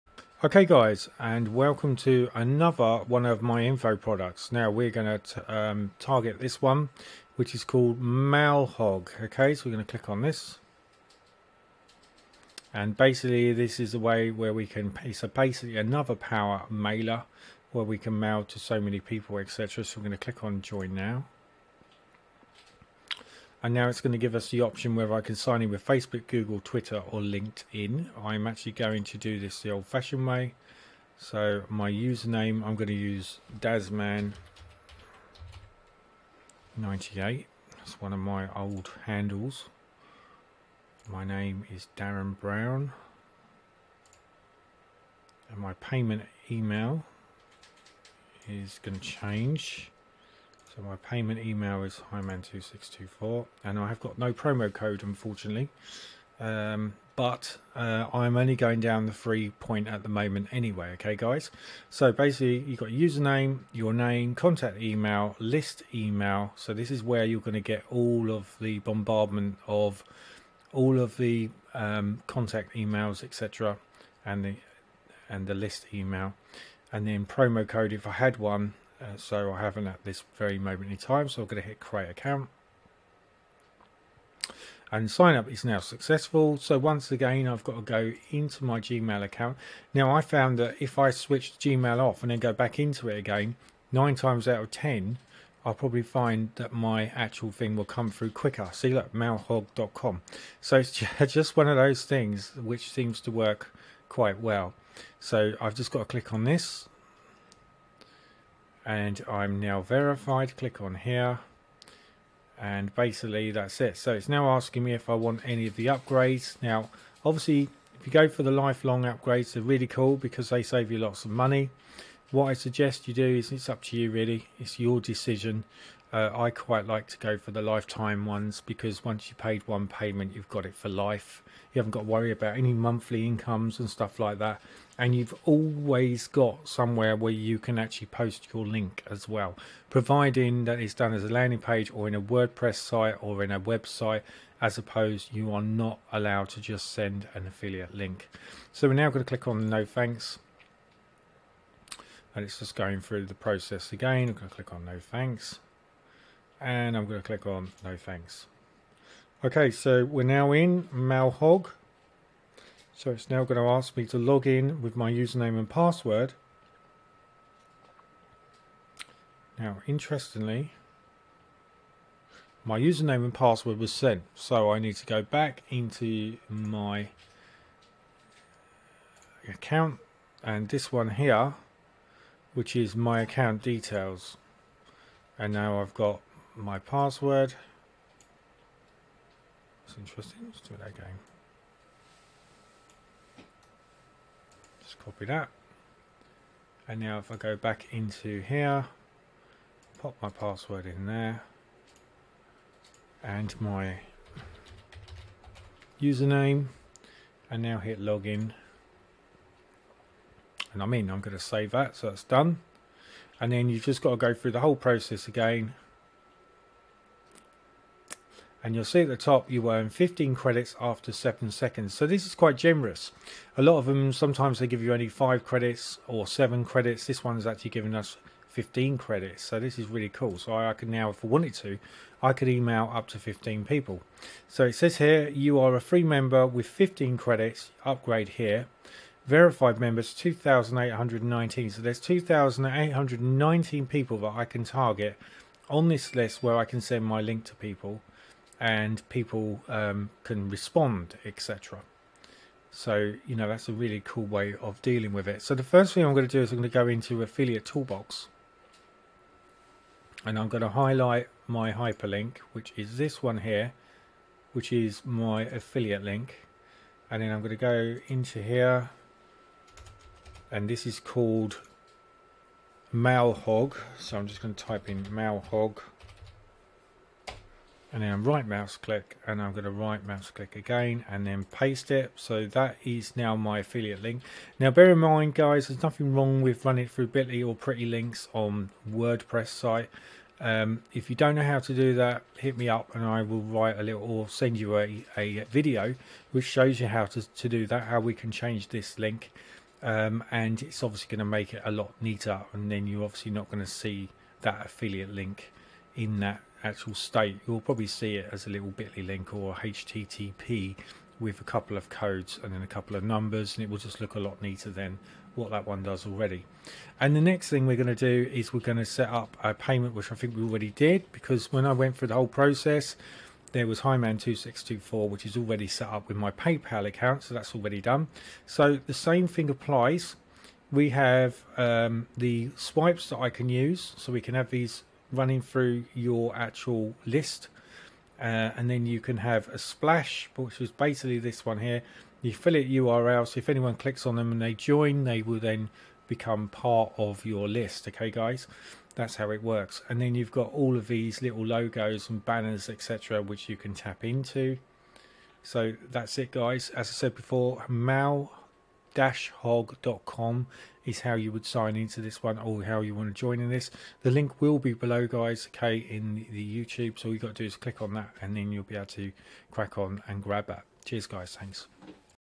Mail Hog Unbelievable way of building and mailing to a list
Mail Hog Unbelievable way of building and mailing to a list with the potential of creating your own email list and followers with long term connections for your own business
Mail Hog Review- Another great mailing list for free Setups a dream easy to use and great response

Mail Hog mailer Review Blowing up like the election

Mail Hog Review- Grow your Email List -Why do you need this

Question have you a List already & How much did it cost you Really?
This list is inexpensive even if you decide to get the upgrades even if you don't you can still use this for free and it will build you a list without the need for a big list

The answer you won't need to pay anything to use this it's completely free but the upgrades will project your commissions forward and build your own list quicker.

💯 Mail Hog Review will help you with creating a list building tool which you can put into action and build your followers leads and subscribers to your chosen website or by a landing page

💯 Mail Hog is a Traffic Exchange, packed with unique features and tools. It is one of the most popular and active traffic exchanges, and also it’s voted the best traffic exchanges by members. This help page is designed to help you use the features of Mail Hog, with information and easy step by step instructions. You can translate the page in the sidebar to the right if it is easier for you.

💯 Link here Gets you free Traffic Now!